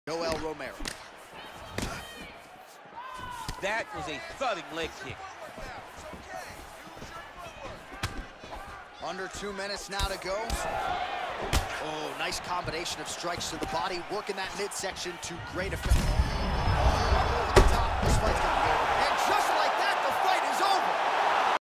Yoel Romero zzzz
Kevin Holland finishes Yoel Romero